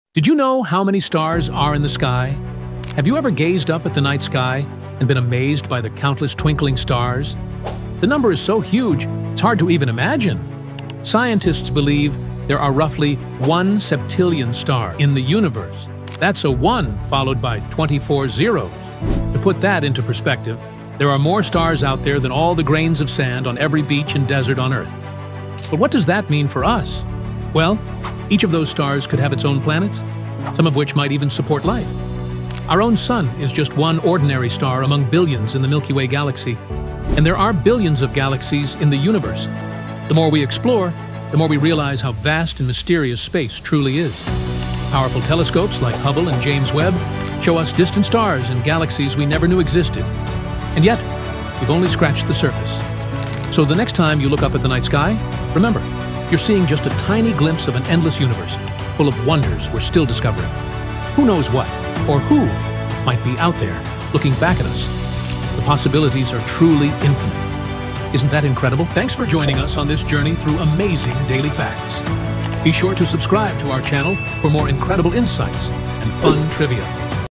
✨ Did You Know There Are 1 SEPTILLION Stars? 🌌
Did you know there are roughly 1 septillion stars in the universe? That’s a 1 followed by 24 zeros! 🌟 To put it in perspective, there are more stars than grains of sand on Earth. Each star could have planets—some might even support life! Our Sun is just one of billions in the Milky Way, and there are billions of galaxies out there. The universe is endless, and we’ve only scratched the surface. 🔭

Next time you gaze at the night sky, remember—you’re seeing a tiny glimpse of an infinite cosmos. Who knows what’s out there? 🌠